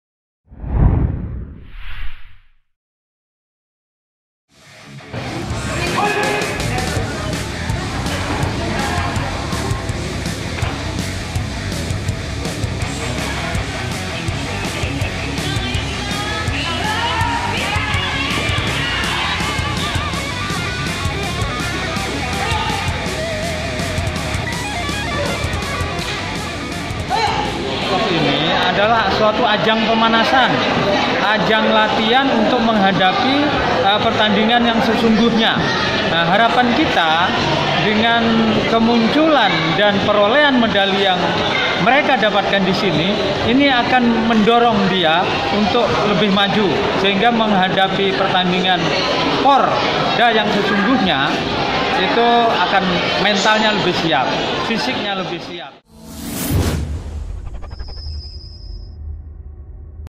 Serunya Kejurprov Taekwondo Praporprov X 2017 Se Kalsel
BANJARMASINPOST.CO.ID, BANJARMASIN - Kejuaraan Provinsi (Kejurprov) Taekwondo Pra Porprov Kalimantan Selatan (Kalsel) telah diselenggarakan pada 23-25 Mei 2017 di Gedung Taekowondo, Banjarmasin, Minggu(25/5/2017).

Sebanyak 154 Taekwondoin berpartisipasi pada gelaran Kejurprov yang diselenggarakan oleh Pengurus Provinsi (Pengprov) Taekwondo Indonesia (TI) Kalsel tersebut.

Pada Kejurprov itu, mempertandingkan kyurogi putra dan putri.

Putra mempertandingkan kelas under 54 kilogram, 58 kilogram, 63 kilogram, 68 kilogram, 74 kilogram, 80 kilogram, 87 kilogram dan 87 kilogram over.

Sedangkan Putri mempertandingkan under 46 kilogram, 49, 53, 57, 62, 67, 73 dan 73 kilogram over. Sedangkan Poomsae yang akan dipertandingkan kelas individual dan pasangan.

Ketua umum Pengprov TI Kalsel, Kolonel Inf (Purn) Slamet Riyanto menuturkan Kejurprov ini sebagai ajang pemanasan sebelum bertanding di Porprov di Tabalong Oktober mendatang.

"Sebagai bentuk latihan atau gladi para atlet sebelum bertanding di arena sesungguhnya nanti, sekaligus dapat mengukur sejauh mana hasil latihan yang telah diterapkan," paparnya.

Slamet menambahkan ajang tahunan Kejurprov ini untuk memacu semangat para Taekwondoin se-Kalsel agar mampu berprestasi lebih baik di tingkat yang lebih tinggi.

 "Dari raihan medali yang mereka peroleh dapat mendorong para atlet dan memotivasi agar lebih maju dalam menghadapi Porprov nanti," tandasnya.

Pasca Kejurprov Pra Porprov, Slamet mengharapkan agar mental bertanding dan fisik para Taekwondoin lebih bagus dan lebih siap dalam menyongsong multi event empat tahunan Porprov. (Banjarmasin Post/Mariana)